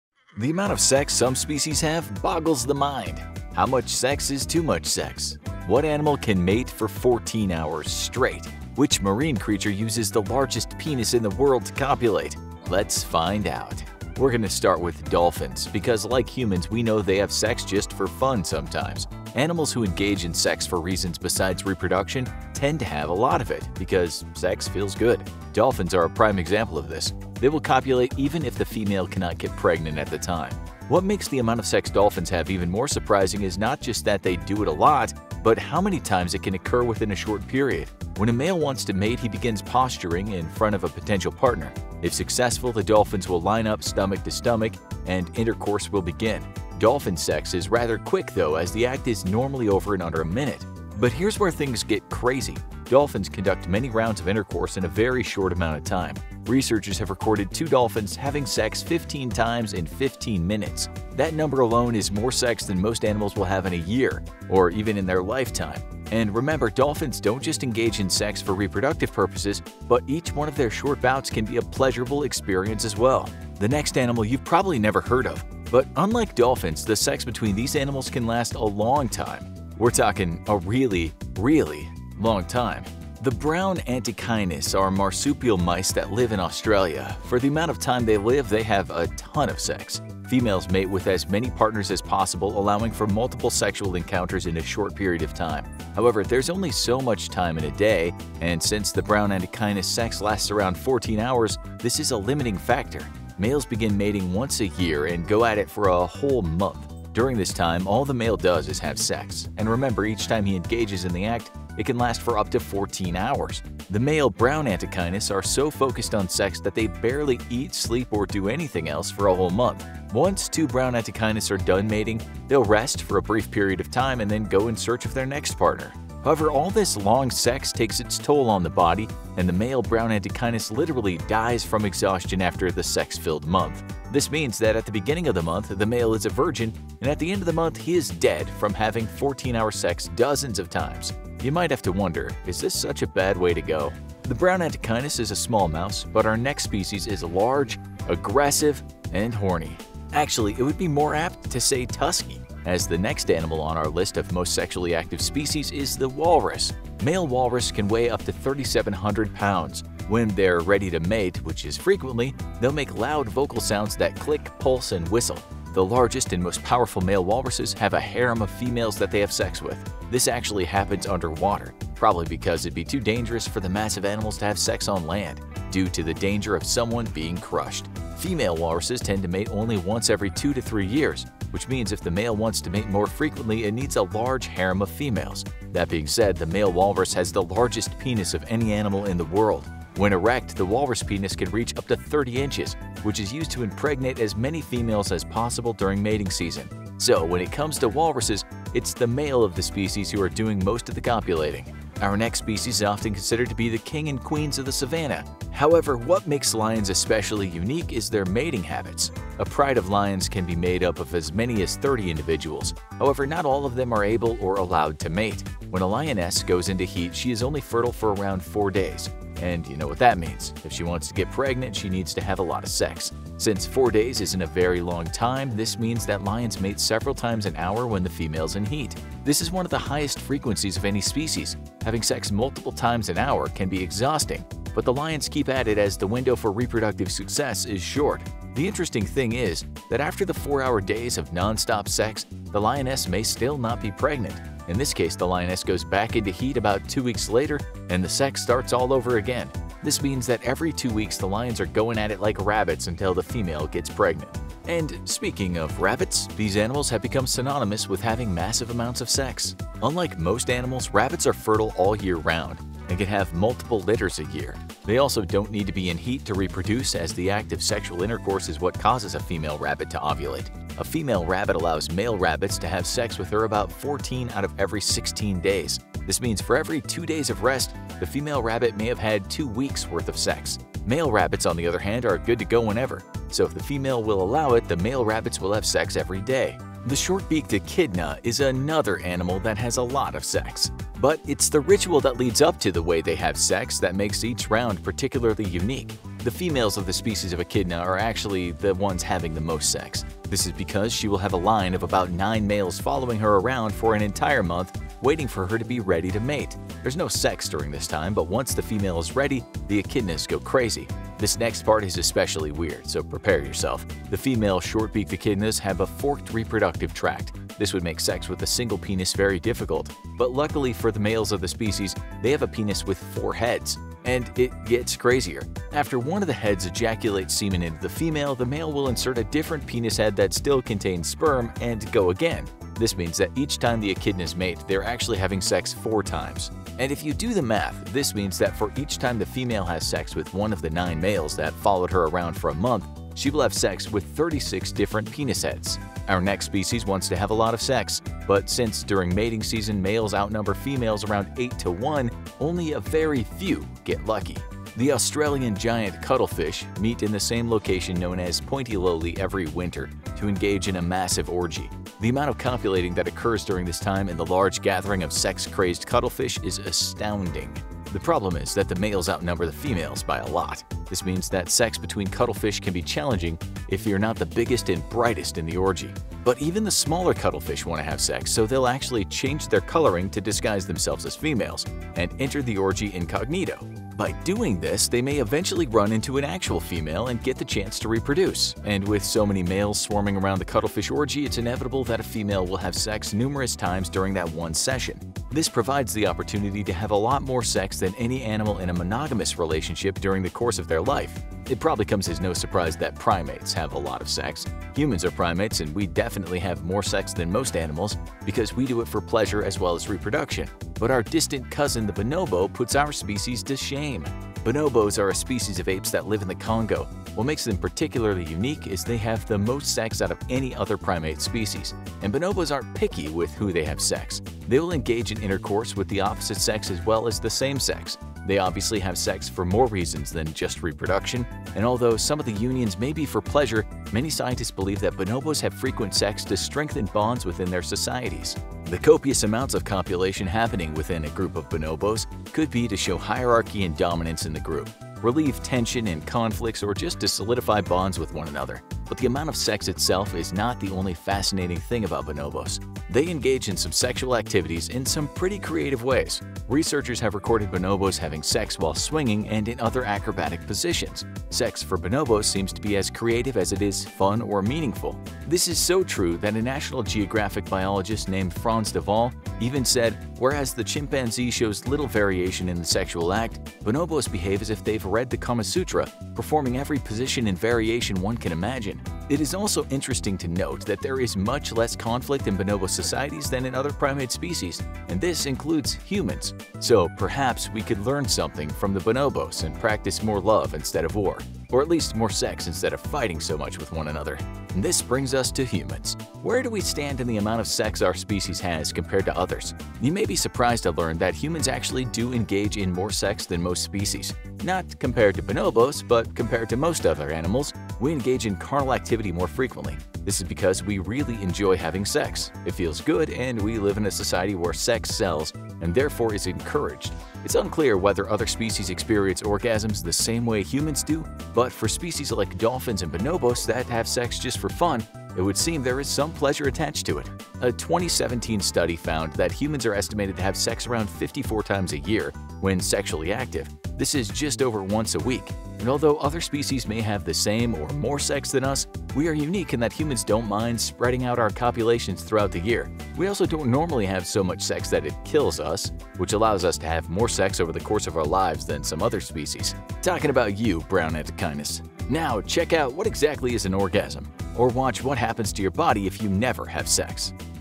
Which Species Has the Most Sex
Procreation is what keeps every species on Earth alive, but some species do it way more than others. Find out where humans rank on the list of the most frisky animals on the planet in today's new video all about sex.